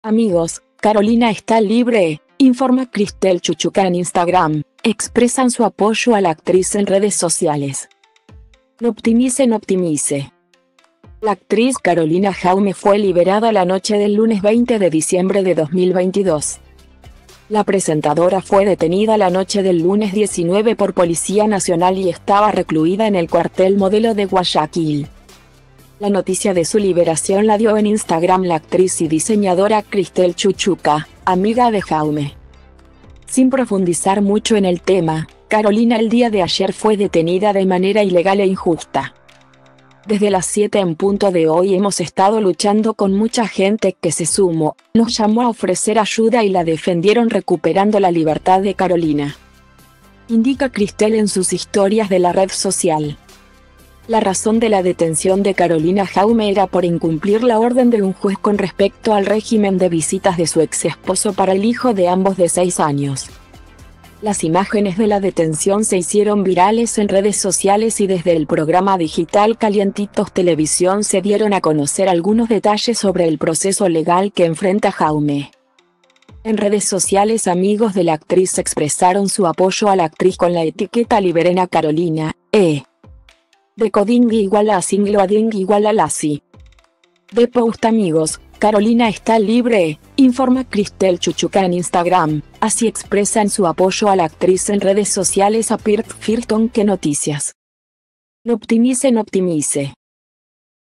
"Amigos, Carolina está libre", informa Krysthel Chuchuca en Instagram: expresan su apoyo a la actri
noptimize/noptimizeLa actriz Carolina Jaume fue liberada la noche del lunes 20 de diciembre de 2022.La presentadora fue detenida la noche del lunes 19 por Policía Nacional y estaba recluida en el Cuartel Modelo de Guayaquil.